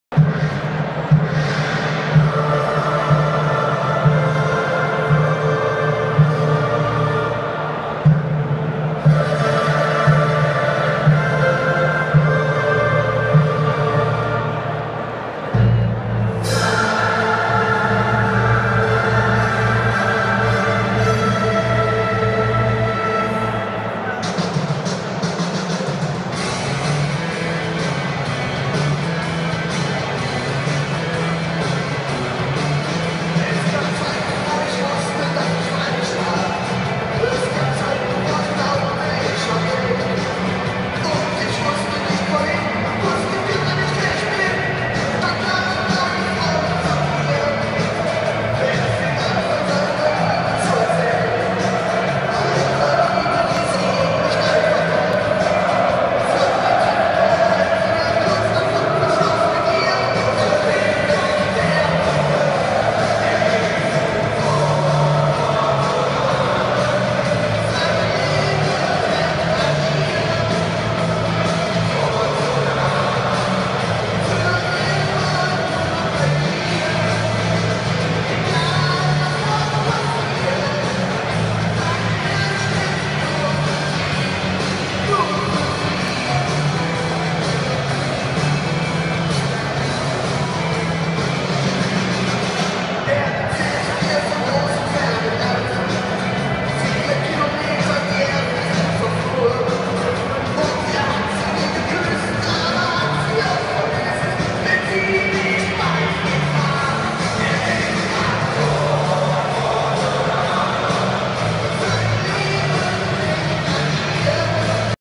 22.08.2015 120 Jahre Fortuna Düsseldorf F95 Choreo Teil 1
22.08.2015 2. Bundesliga 13.00 Uhr
Fortuna Düsseldorf vs SC Freiburg 1:2